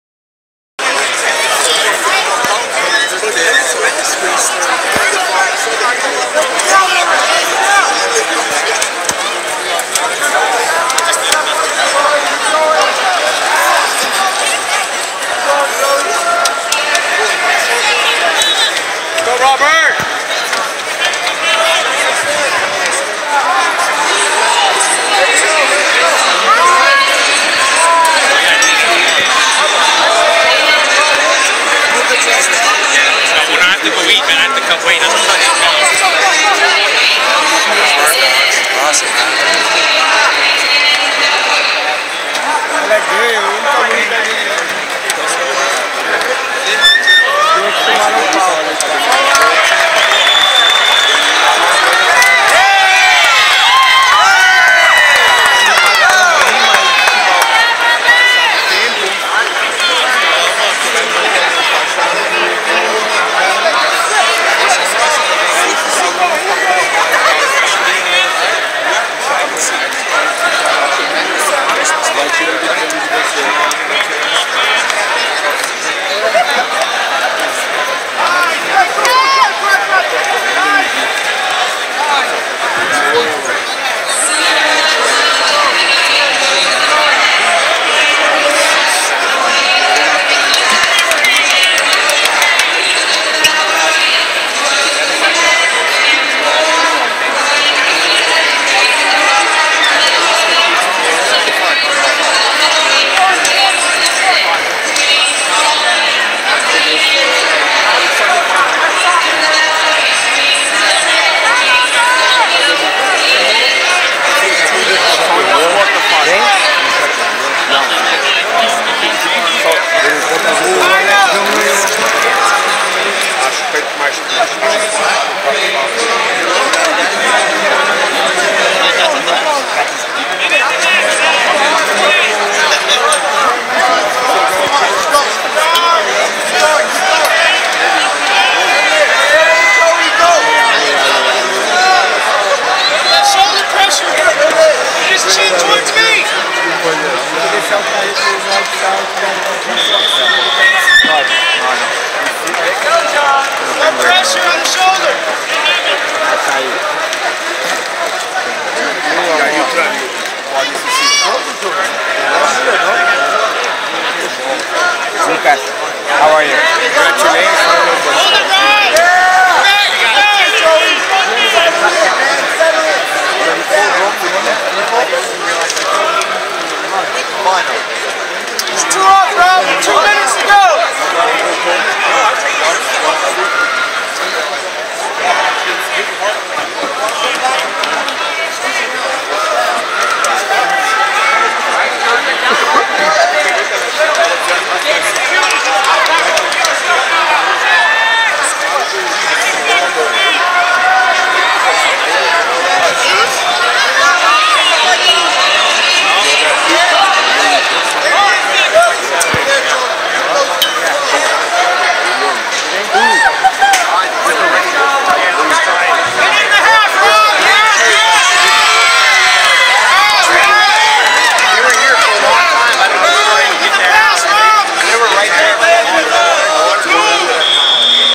Joey Alvarado Fight #2 IBJJF Master/Senior Worlds 2013
Creator of Kettle-Jtisu and Owner/Master trainer of SoCal mma and fitness on his way to the silver medal in the 2013 Ibjjf Master/Senior World Championships.